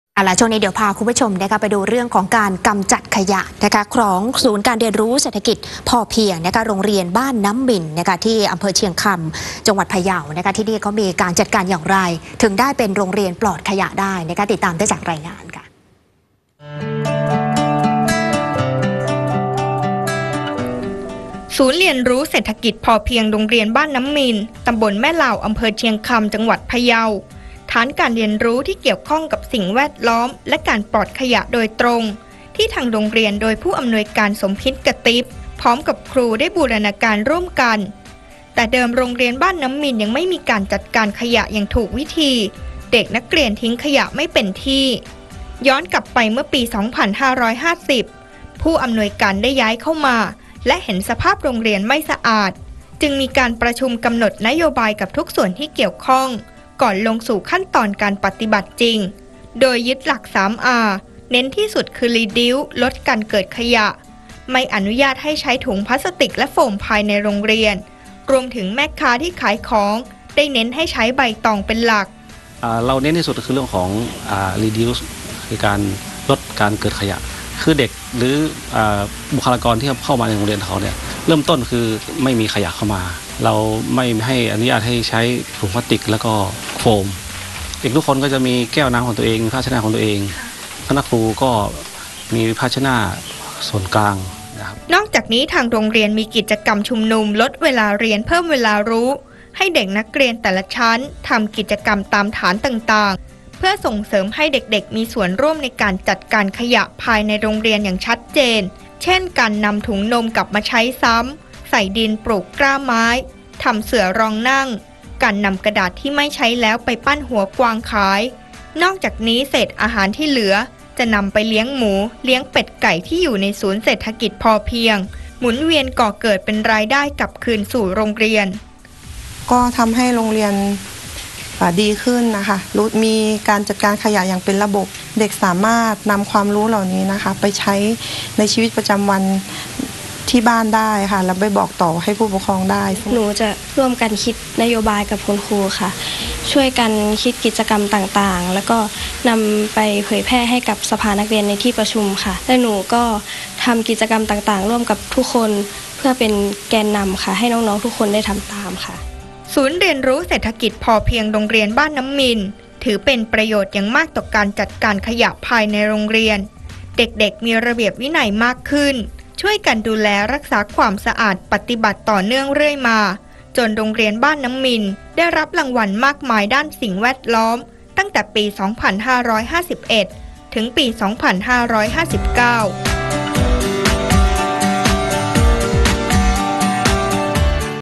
โรงเรียนบ้านน้ำมิน โรงเรียนปลอดขยะ | 9 เม.ย.61 | ข่าวจริง
ศูนย์เรียนรู้เศรษฐกิจพอเพียง โรงเรียนบ้านน้ำมิน ตำบลแม่ลาว อำเภอเชียงคำ จังหวัดพะเยา ฐานการเรียนรู้ที่เกี่ยวข้องกับสิ่งแวดล้อม และการปลอดขยะโดยตรง ที่ทางโรงเรียน โดยผู้อำนวยการสมพิศ กาติ๊บ พร้อมกับครูได้บูรณาการร่วมกัน แต่เดิมโรงเรียนบ้านน้ำมินยังไม่มีการจัดการขยะอย่างถูกวิธี